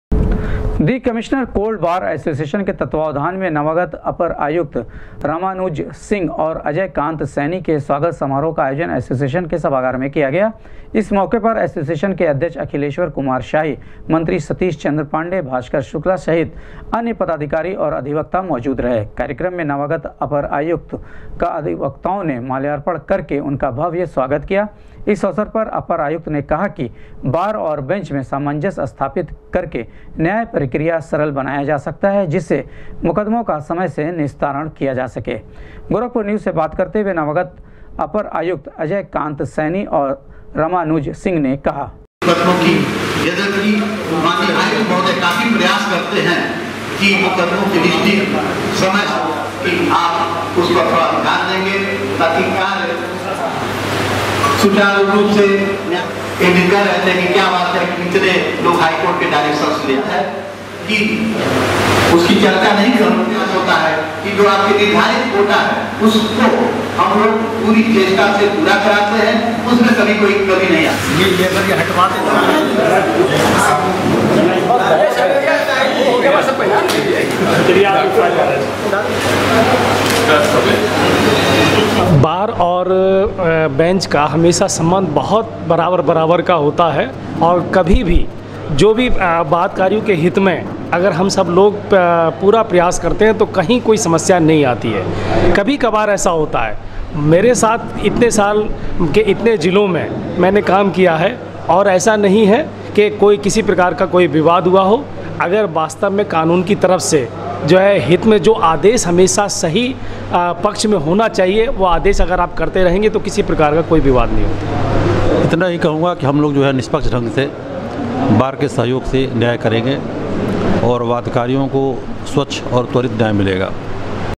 दि कमिश्नर कोर्ट बार एशोसिएशन ने किया नवागत अपर आयुक्त का स्वागत, कहा बार और बेंच
दि कमिश्नर कोर्ट बार एशोसिएशन ने किया नवागत अपर आयुक्त का स्वागत, कहा बार और बेंच में सामन्जस्य स्थापित करके किया जाएगा मुकदमों का निस्तारण